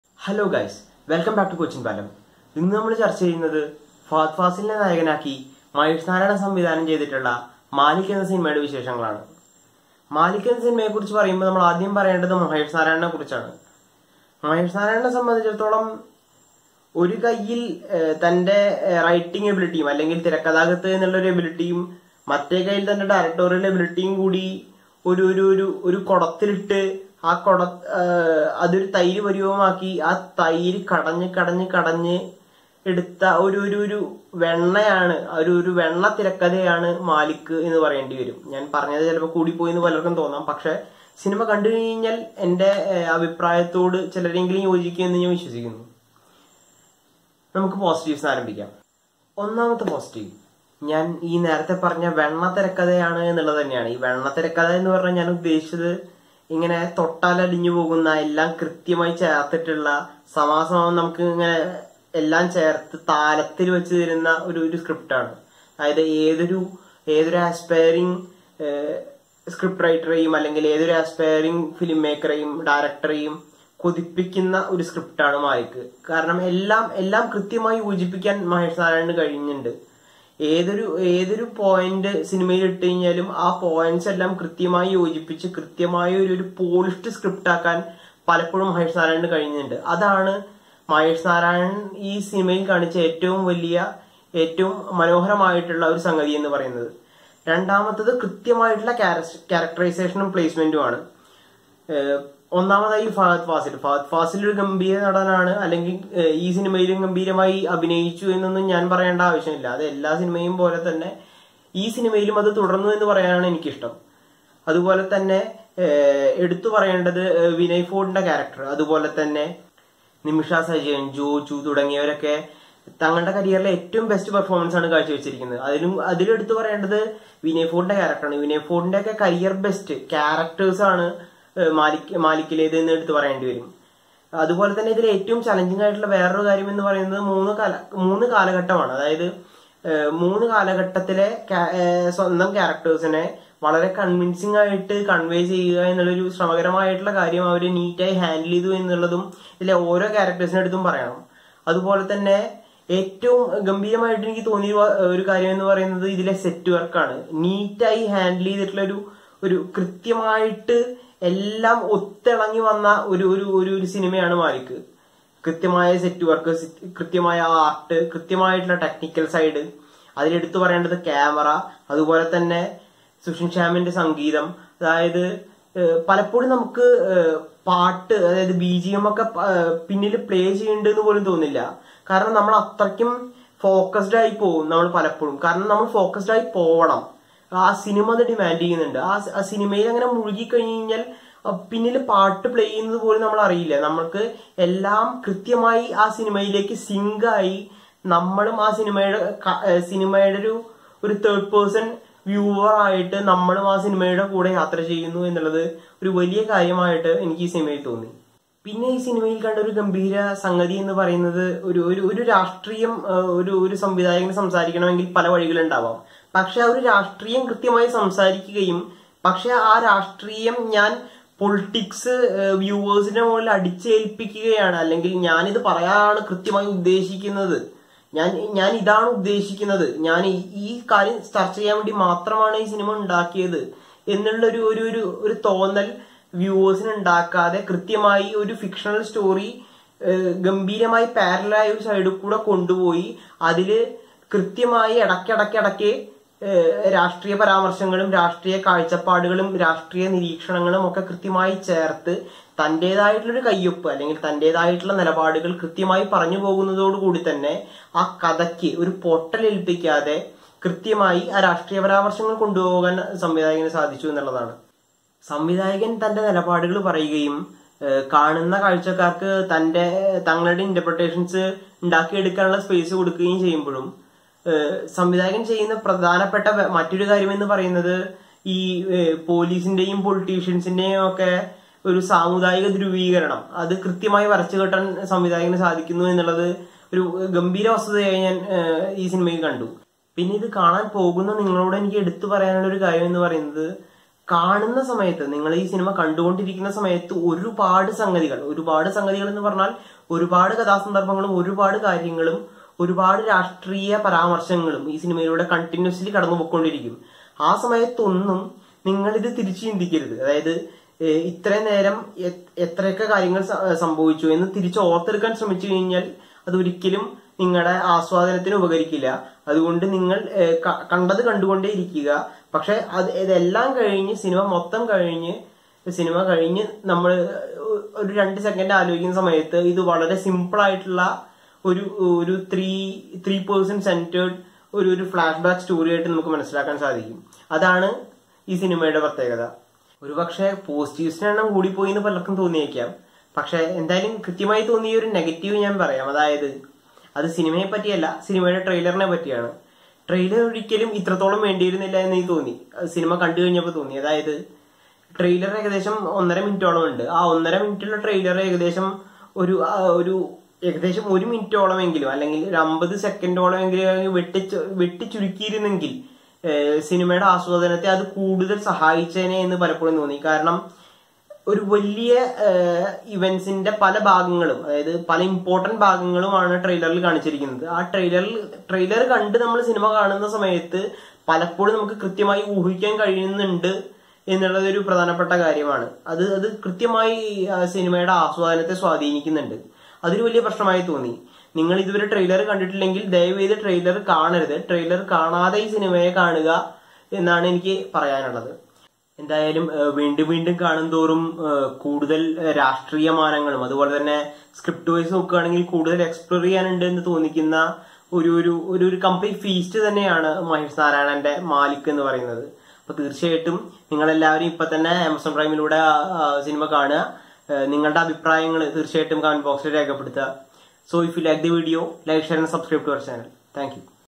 Malik Malayalam Movie Review | Mahesh Narayanan | Fahad Fasil | Amazon Prime | Kochin Palam
Malik is a new malayalam movie directed by Maheshnarayanan Staring Fahadfasil as lead role.

consumer electronics